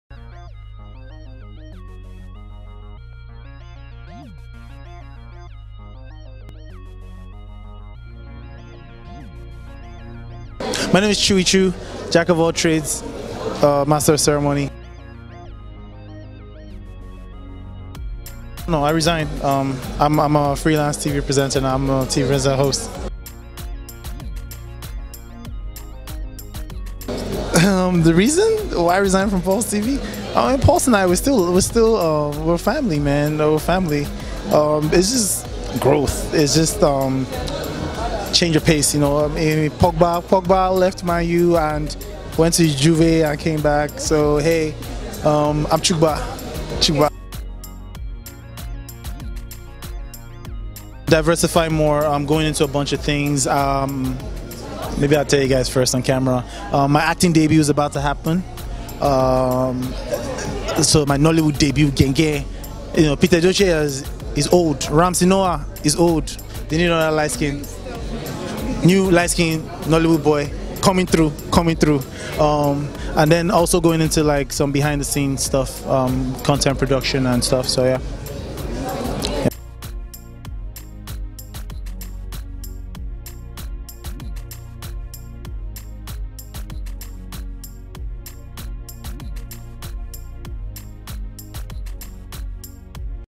Chuey Chu talks about his exit from Pulse TV
Chuey Chu is a TV presenter popularly known as "The Pulse guy" from his show "Pulse TV Strivia".

The presenter who joined Pulse TV in may 2015 and took several media by storm with his hilarious show has recently resigned. He talks about his exit, his acting debut and more in this exclusive interview with Broadway TV.

Broadway TV is the TV arm of the popular Nigerian blog, It is entertainment, people and lifestyle-focused channel.